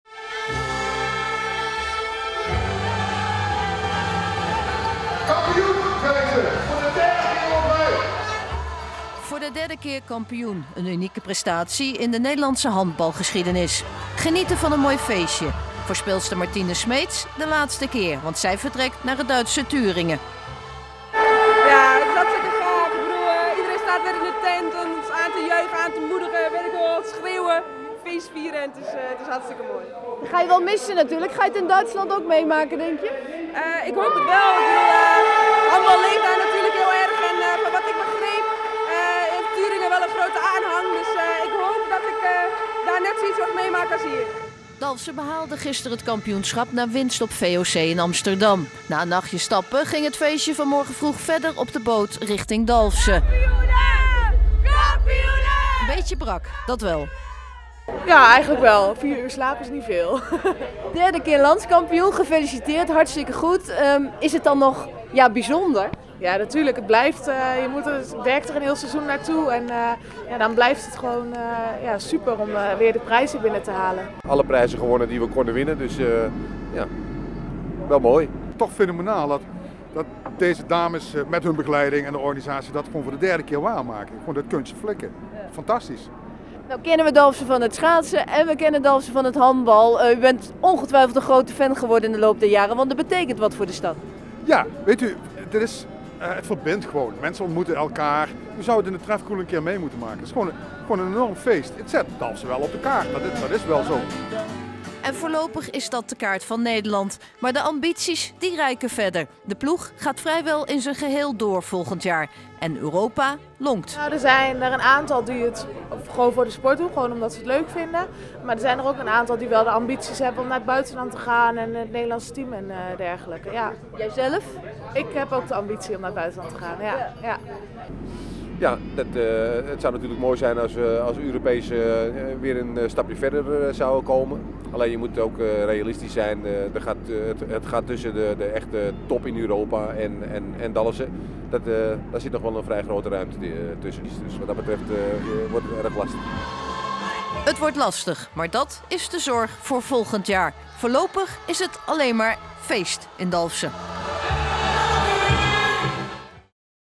Handbalsters Sercodak in Dalfsen gehuldigd
De handbalsters van Sercodak Dalfsen zijn zondag in Dalfsen gehuldigd voor het behalen van het landskampioenschap. Ruim 300 fans hadden zich langs de havenkade verzameld om de dames, die per boot in Dalfsen aankwamen, een warm welkom te geven.

Even later ging het feest los in de grote tent achter het gemeentehuis. De prestatie is bijzonder. De laatste twintig jaar is het geen enkele andere dames handbalclub gelukt om drie keer achter elkaar landskampioen te worden.